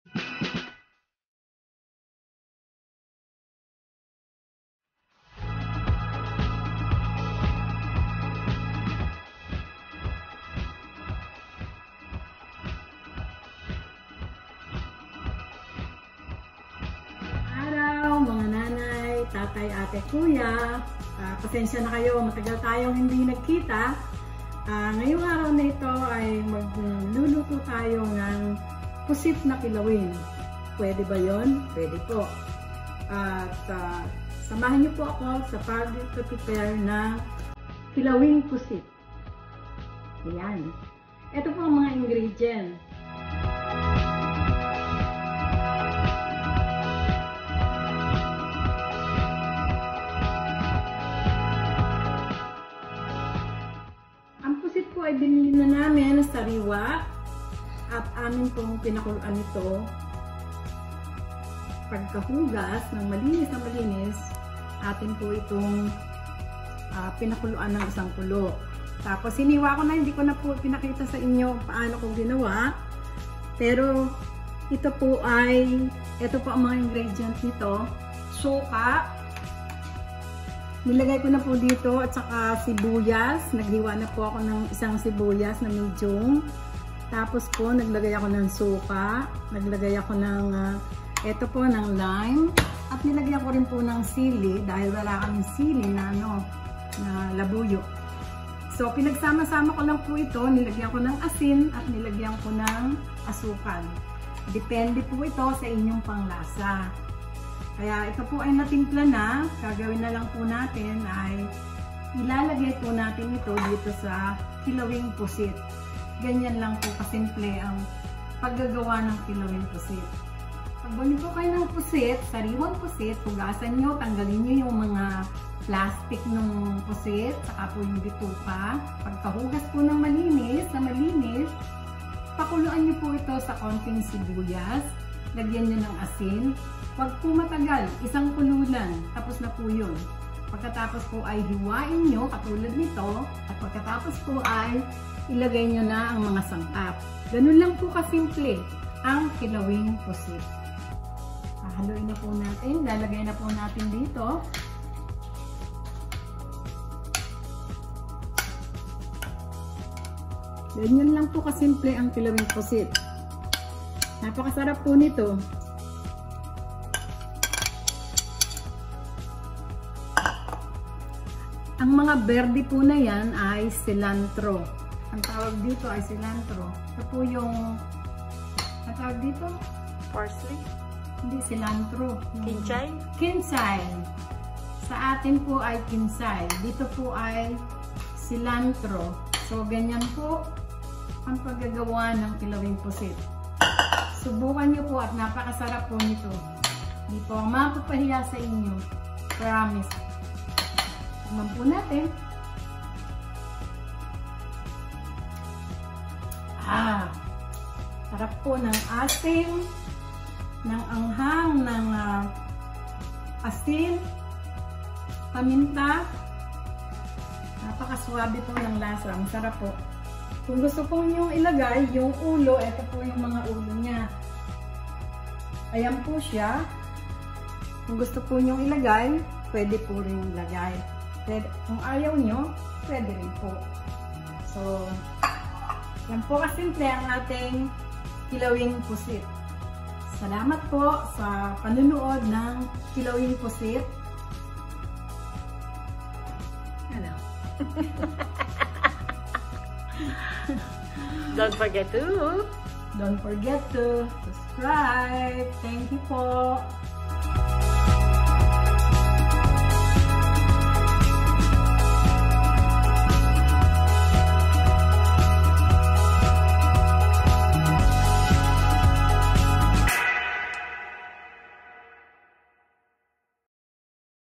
Squid Ceviche or Kilawing Pusit
1 lbs squid
1/2 vinegar
1 tbsp lime or lemon juice
Cilantro
2 tbsp sugar
Red chili peppers or Sriracha Sauce
Salt & Pepper